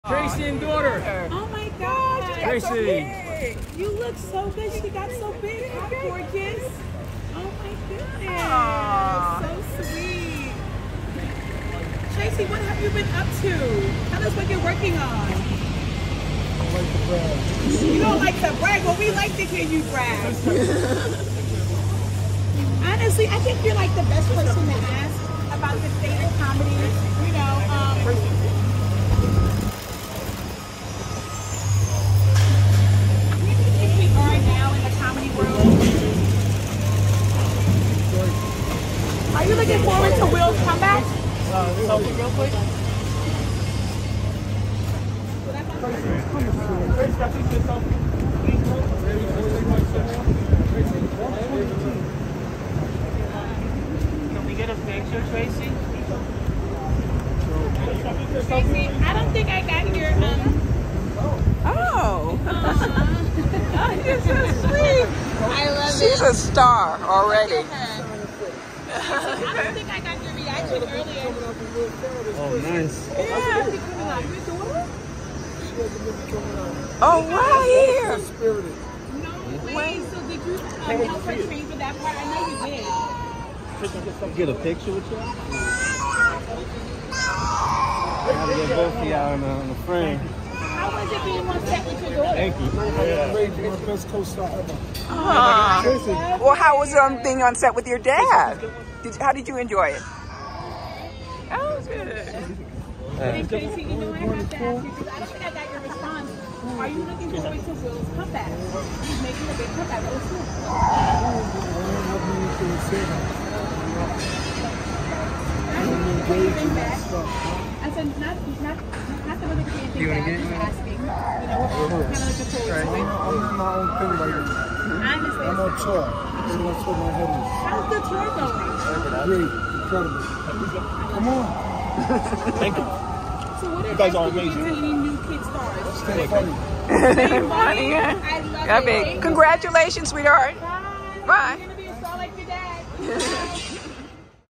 Doting Dad Tracy Morgan and Daughter (and co-star) Maven Visit the Kelly Clarkson Show in NYC
Comedian Tracy Morgan and his adorable daughter Maven stopped by the Kelly Clarkson Show on location in NYC. And Maven handled the paparazzi and fans like a pro.

Morgan and Maven recently co-hosted the Shark Week special Tracy Morgan Presents: Sharks! with Tracy Morgan.